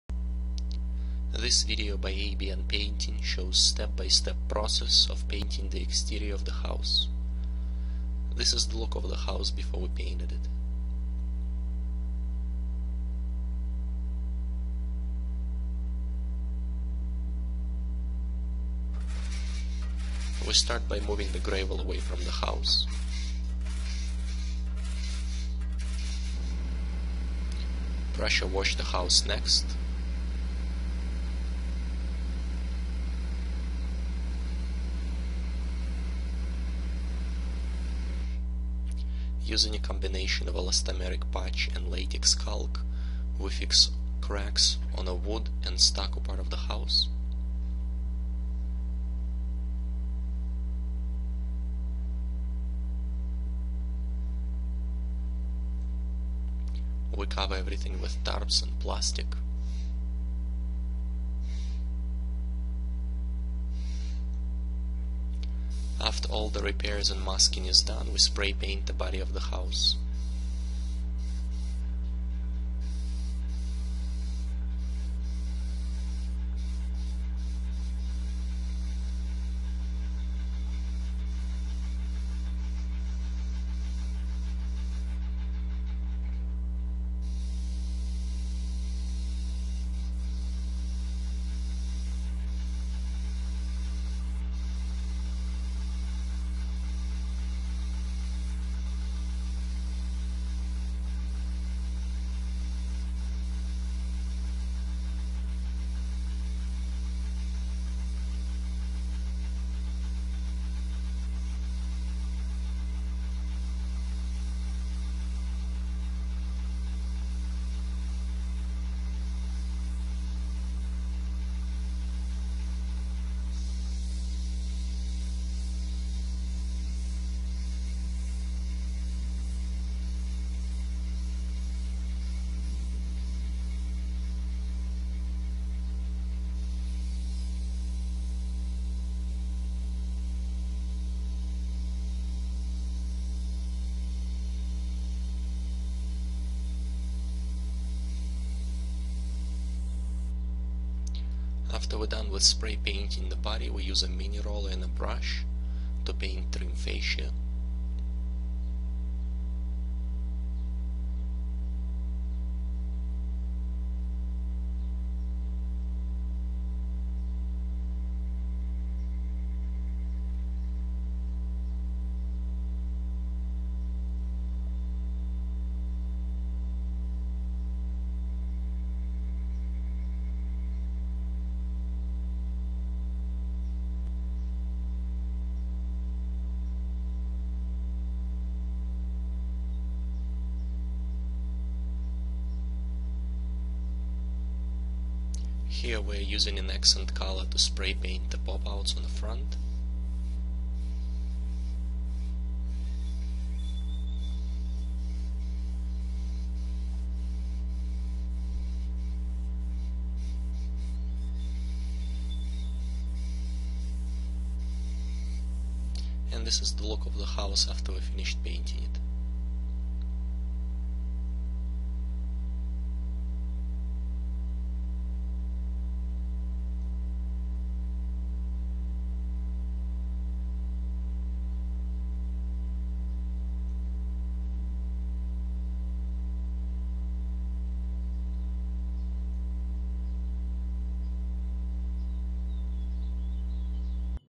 Exterior house preparation and spray painting.
Painting a  house exterior using an airless sprayer to spray paint the stucco body of the house and using a mini roller and a brush to paint wood trim fascia.